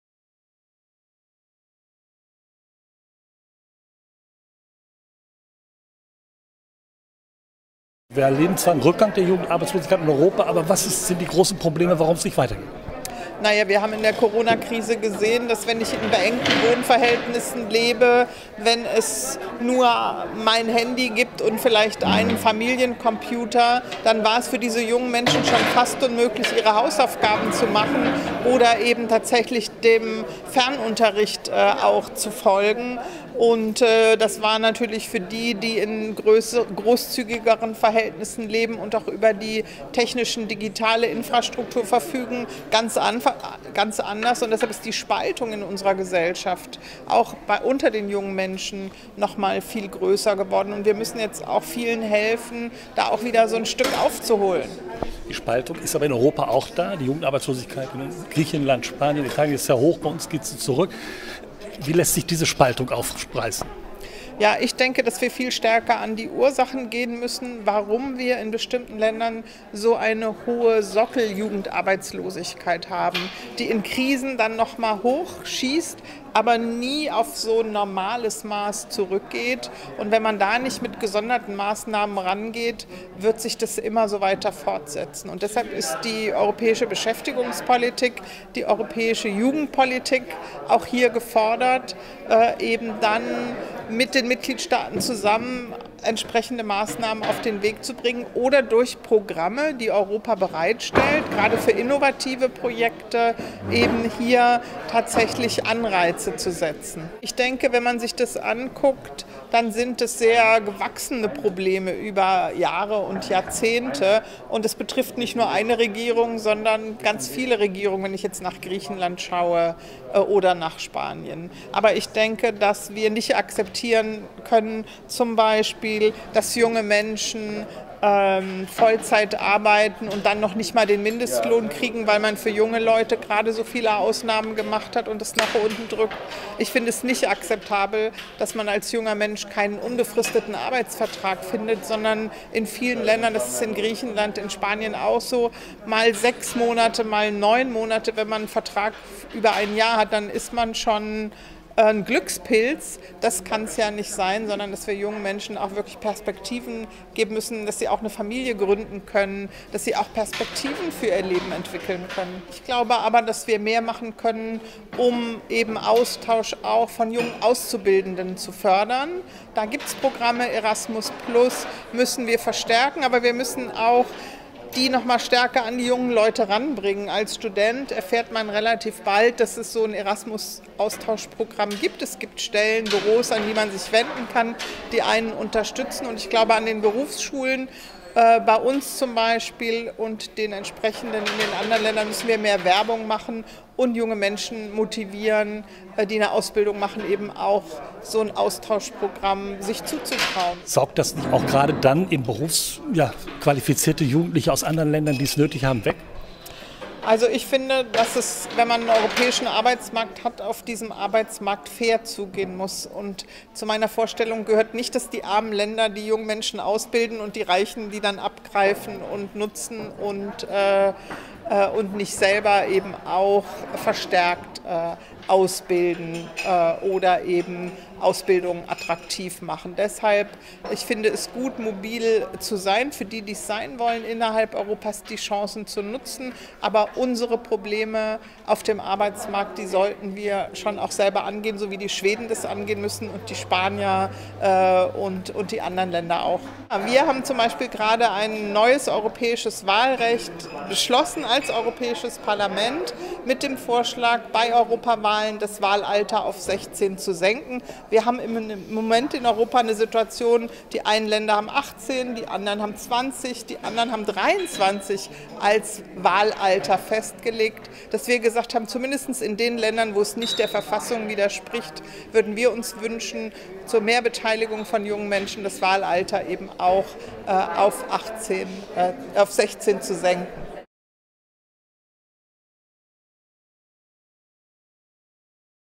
Jugendarbeitslosigkeit in Europa und Corona
Im Rahmen der Kölner Europagespräche ein Interview mit Gabriele Bischoff, S&D, Mitglied des Europa-Parlaments